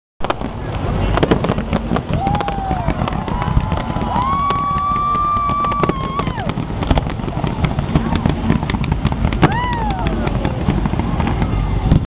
fireworks
fireworks at darling harbour new years eve 2007